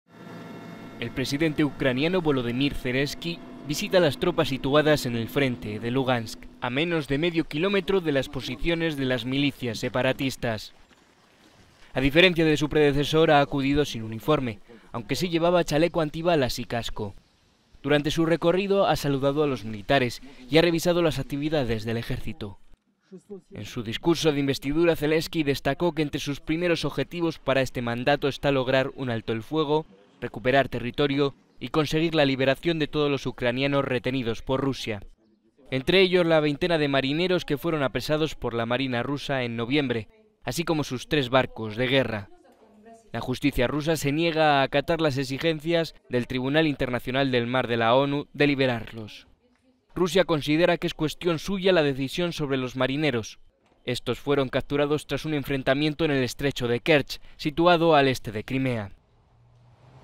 El nuevo presidente de Ucrania visita el frente del este de Ucrania
Durante su recorrido ha saludado a los militares y ha revisado las actividades del Ejército. En su discurso de investidura, Zelenski destacó que entre sus primeros objetivos para este mandato está lograr un alto el fuego, recuperar territorio y conseguir la liberación de los presos de guerra.…